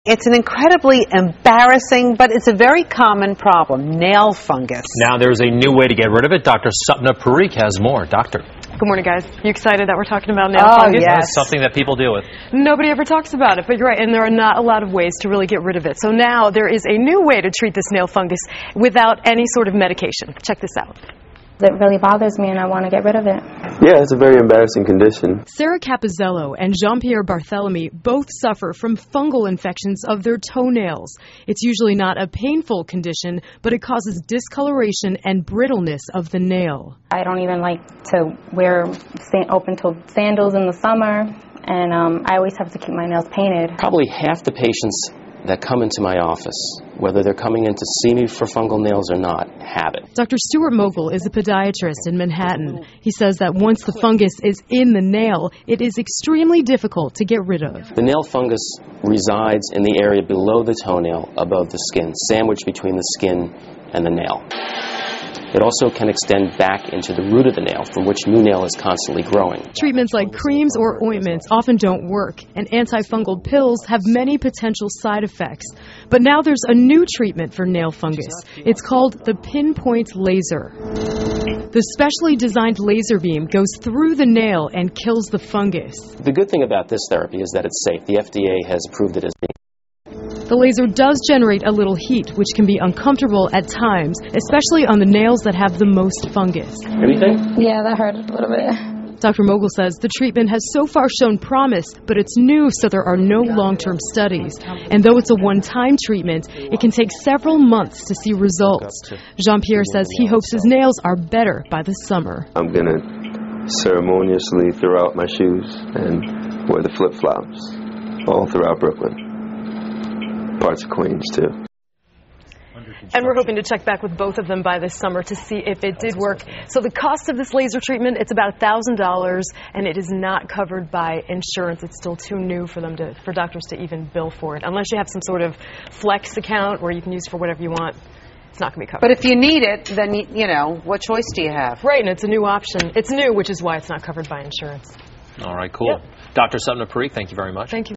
Laser Treatment for Fungal Toenails - Fox 5
Patented PinPointe™ FootLaser™ Treatment for Fungal nails (toenails) Available in two convenient locations in Manhattan at Downtown Podiatry